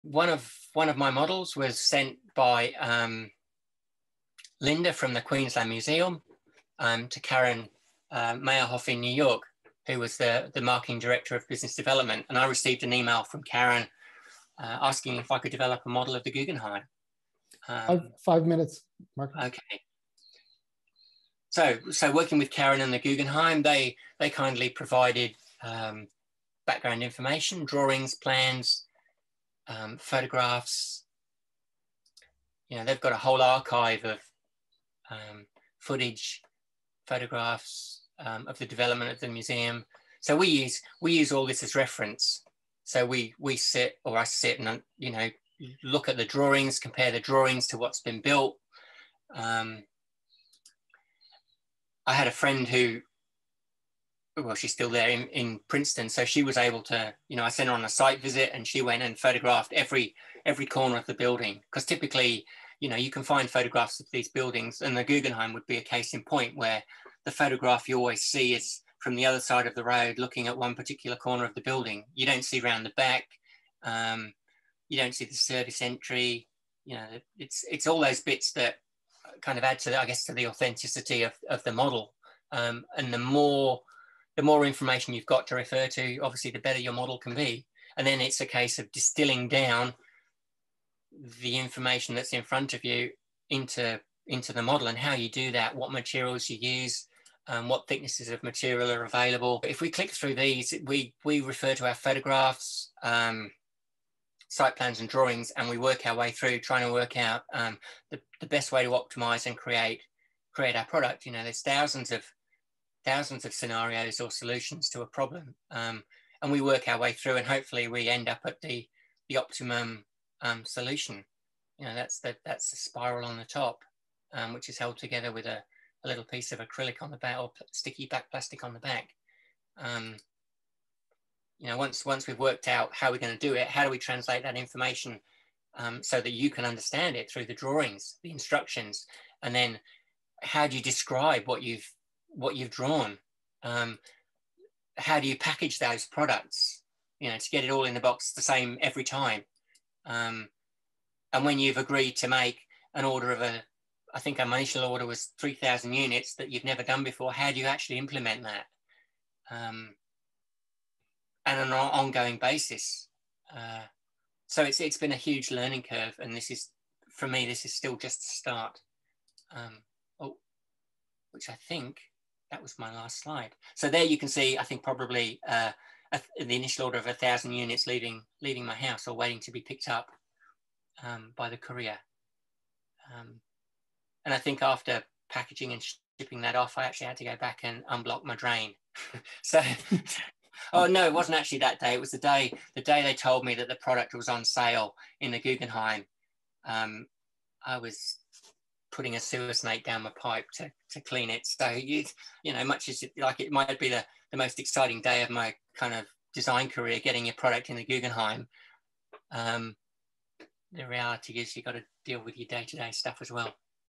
Developing the Guggenheim Museum Scale Model
Marcus shares the story behind Little Building Co creating the Guggenheim Museum Scale Model.